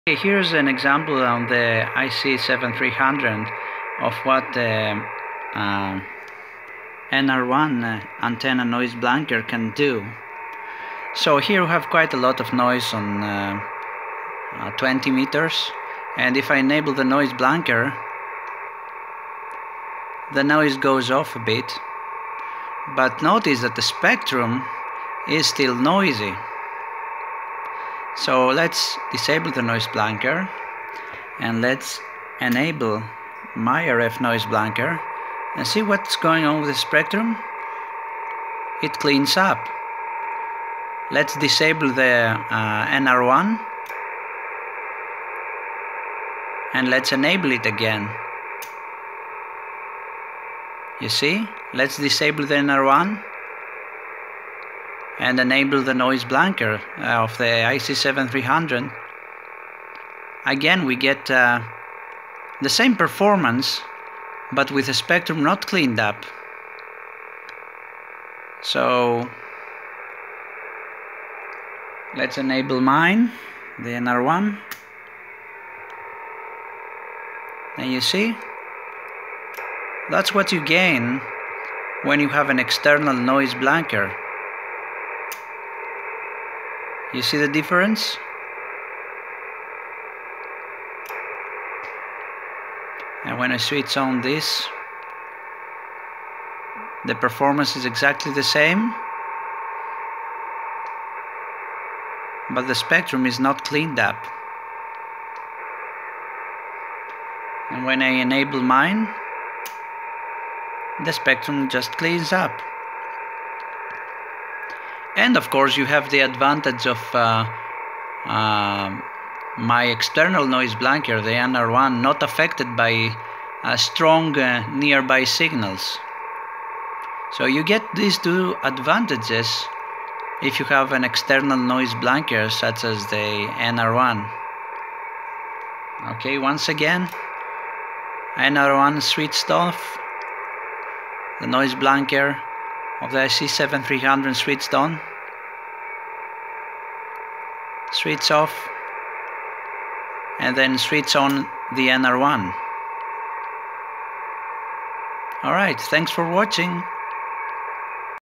NR-1 cleaning the waterfall on IC-7300
This video shows the performance of the NR-1 noise blanker ) on 20m on cleaning the waterfall of modern radios such like the IC-7300, in comparison with the internal noise blankers of those radios.